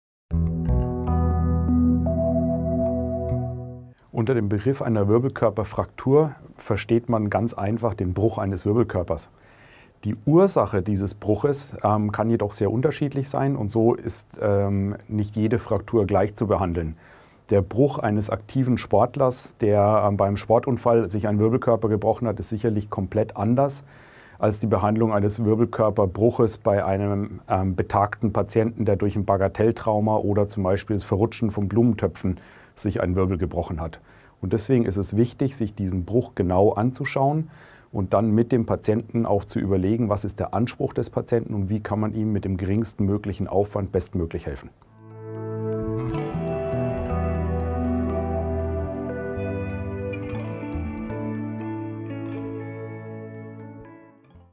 Was ist ein Wirbelkörperbruch? - Medizin ABC | Asklepios
Eine Wirbelkörperfraktur tritt häufig bei älteren Menschen mit Osteoporose auf. Die Stabilität der Knochen lässt im Alter nach und Wirbel können ohne große Krafteinwirkung in sich zusammenbrechen. Glücklicherweise muss aber nicht jeder Wirbelkörperbruch operiert werden. Dr. Robert Morrison ist Chefarzt am Zentrum für Wirbelsäulen- und Skoliosetherapie in Bad Abbach und erklärt, was eine Wirbelkörperfraktur ist.